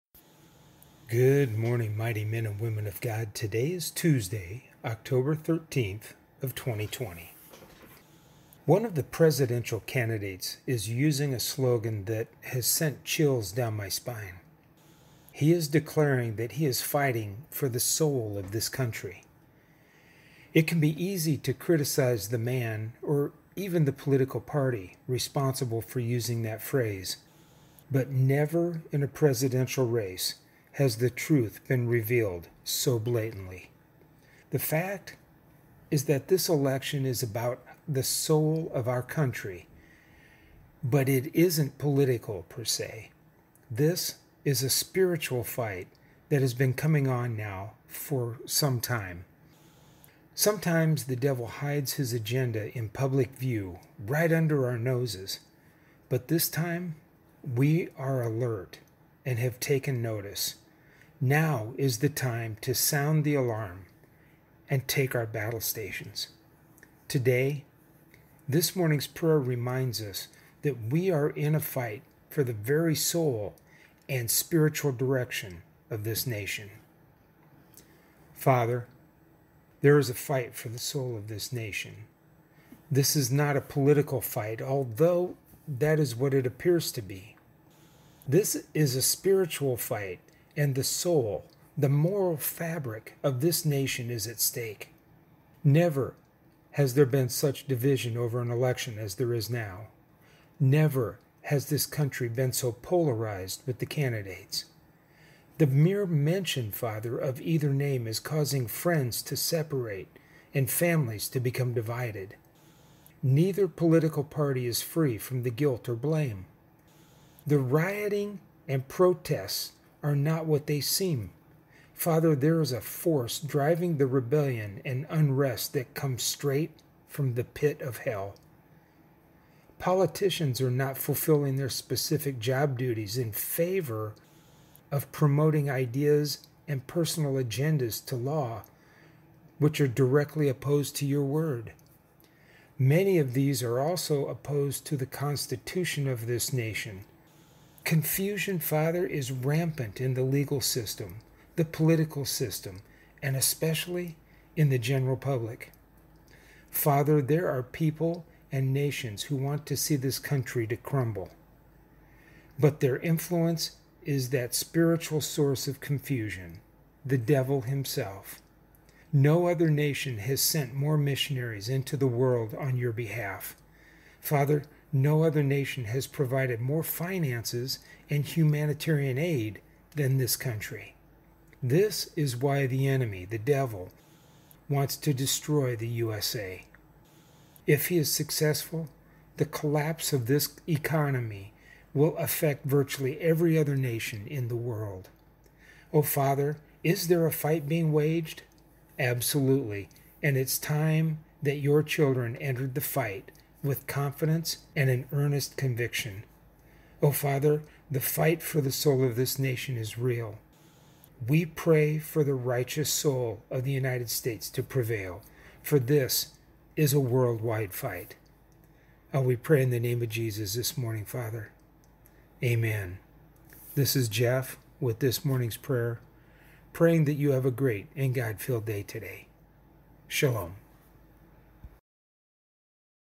Fighting For The Soul Of This Nation!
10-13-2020  This Morning's Prayer for Tuesday
One of the presidential candidates is using a slogan that has sent chills down my spine.  He is declaring that he is fighting for the soul of this country.

It can be easy to criticize the man, or even the political party, responsible for using that phrase, but never in a presidential race has the truth be revealed so blatantly.

The fact is that this election is about the soul of our country, but it isn’t political, per se.  This is a spiritual fight that has been coming on for some time now.

Sometimes the devil hides his agenda in public view, right under our noses.  But this time, we are alert and have taken notice.  Now it is time to sound the alarm and take our battle stations.

Today, This Morning’s Prayer reminds us we are in a fight for the very soul and spiritual direction of this nation!